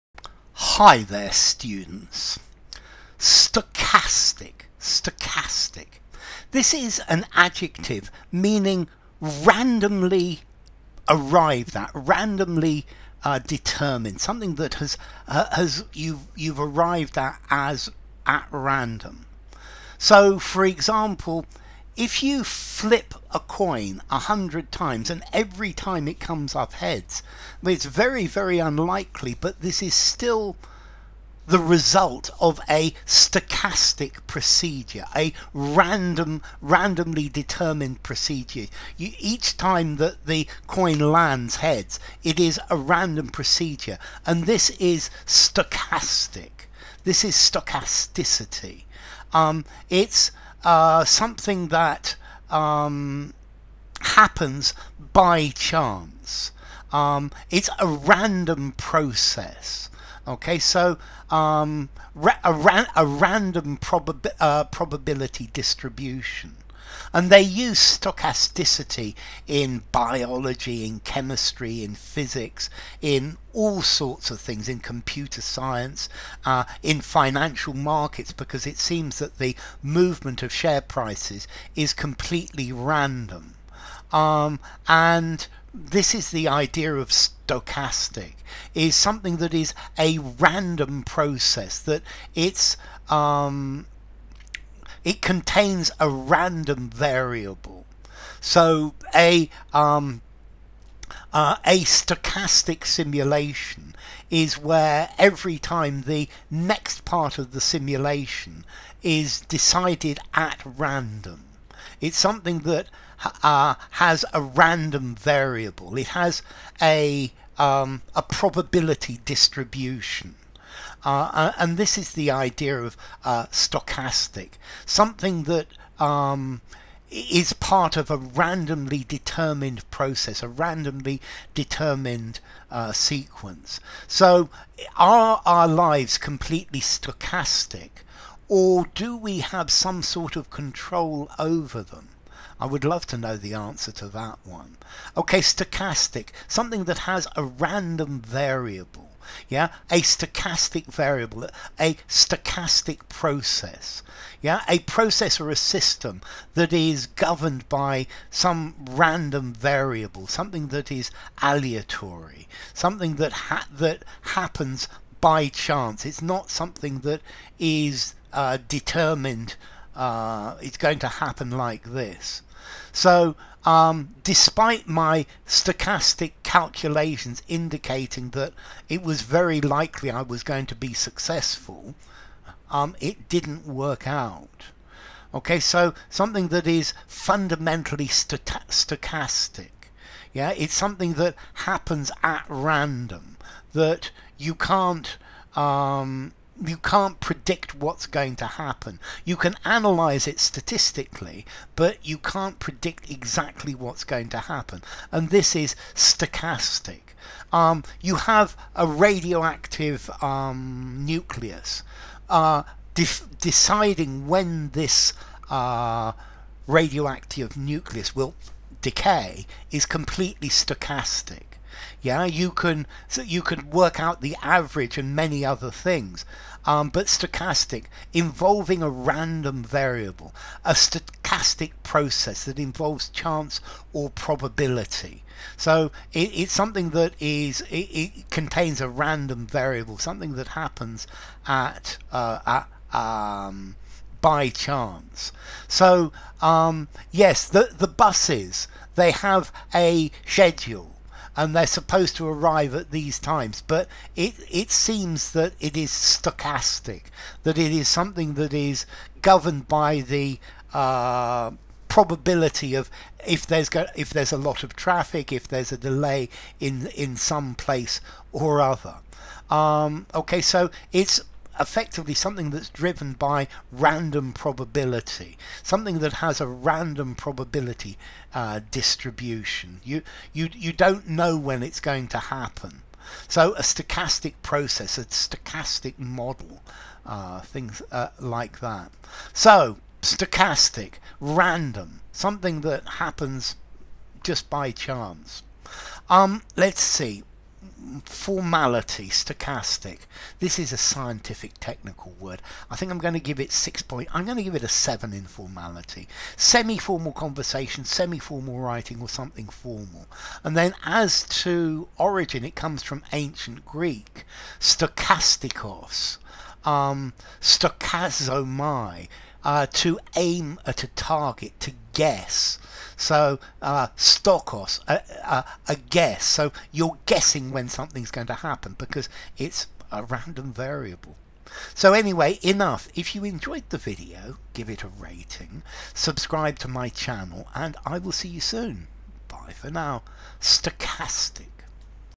😎 Stochastic Meaning - Stochastic Defined - Stochastic Examples - Stochastic Definition - Stochastic
What does stochastic mean? What is stochastic? What is the meaning of stochastic? A definition of stochastic. The video gives various example sentences of stochastic in context for you to be able to use stochastic correctly when speaking and writing.